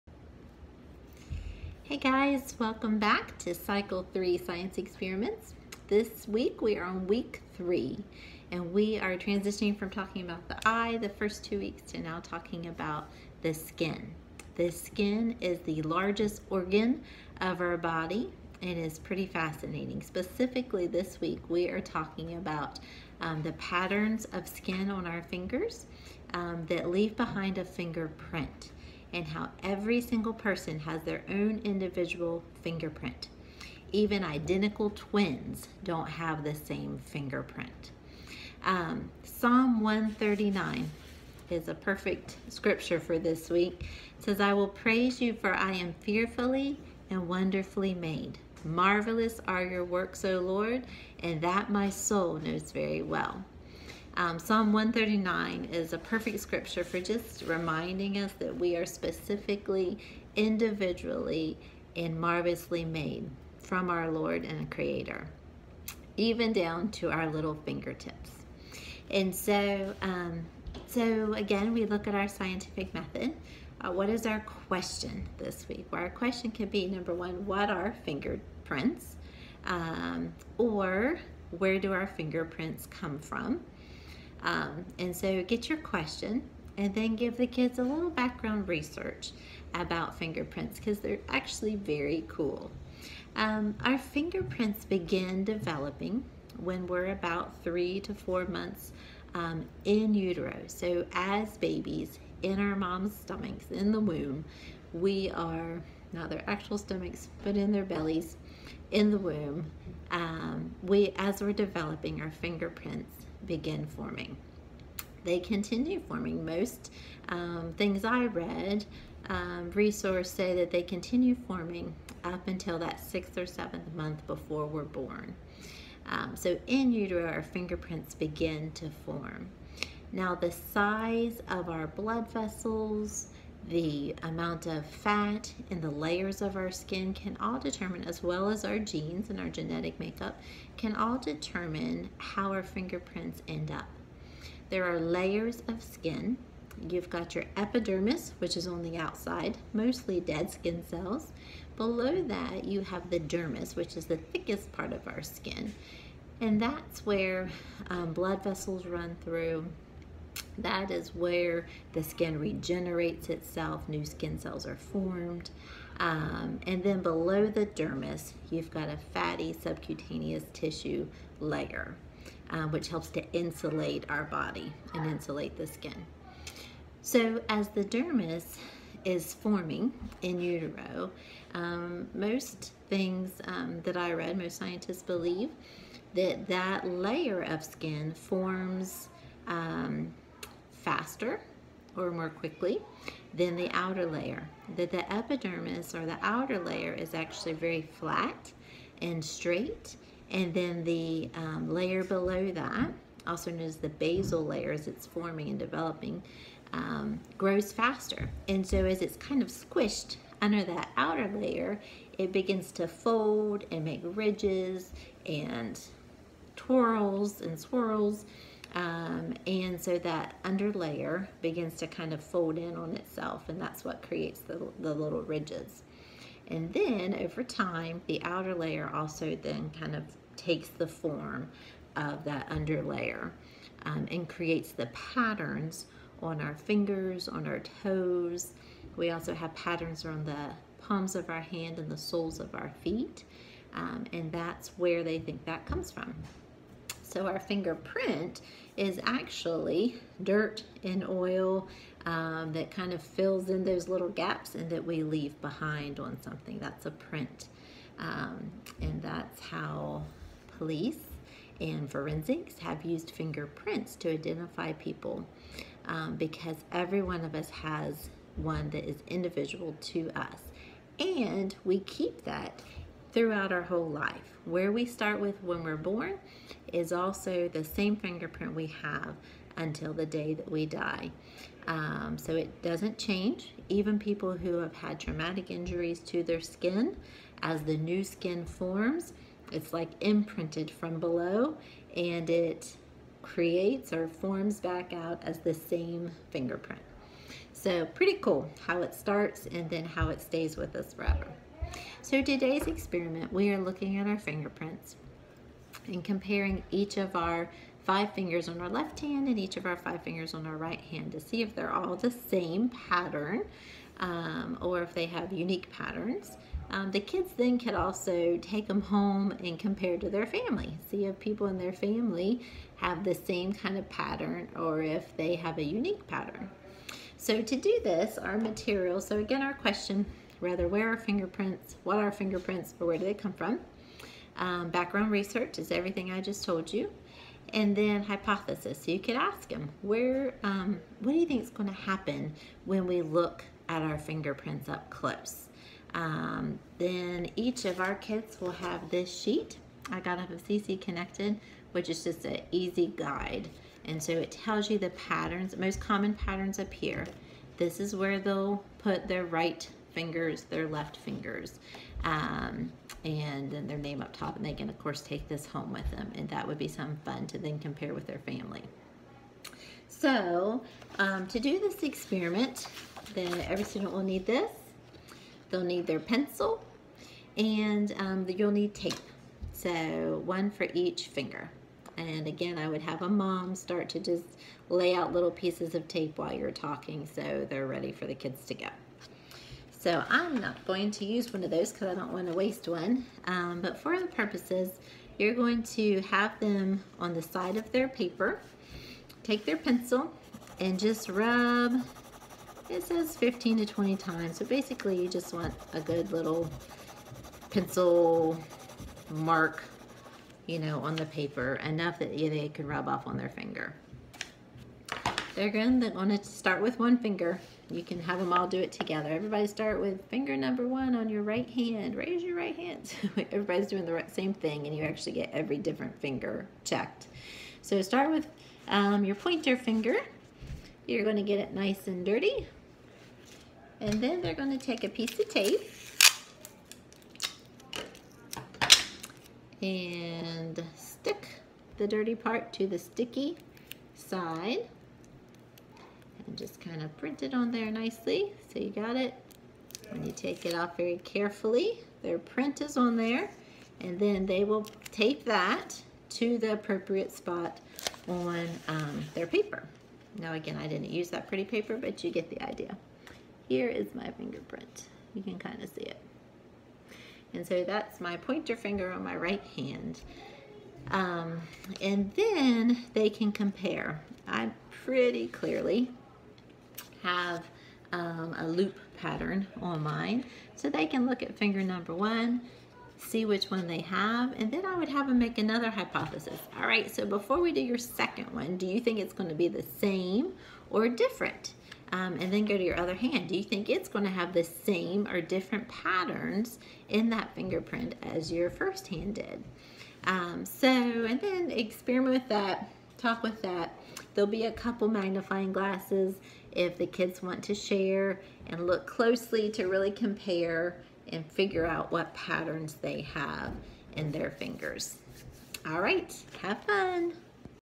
Classical Conversations Cycle 3 Week 3 science experiment
Fingerprints and patterns: Where do they come from?